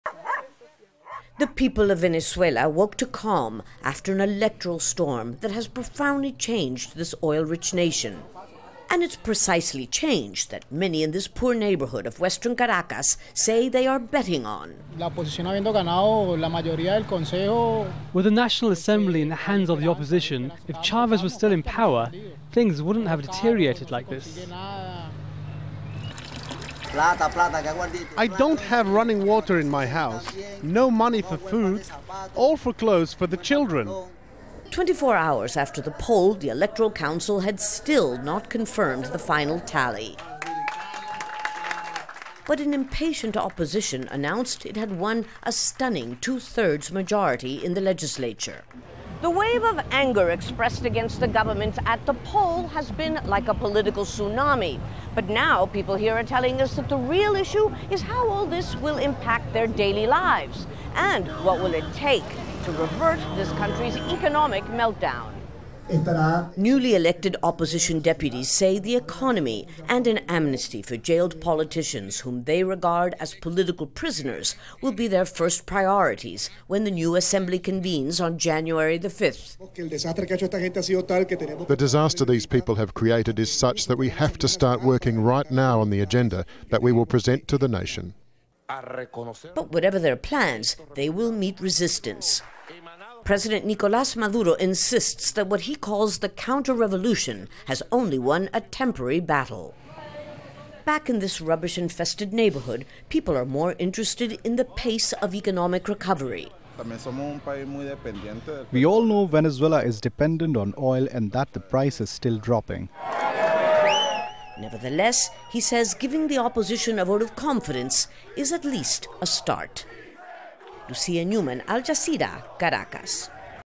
Venezuelan opposition claims rare victory
Voters dismiss the party of Hugo Chavez and Nicolas Maduro, handing the opposition a resounding victory and marking a major political shift.

Al Jazeera's Lucia Newman reports.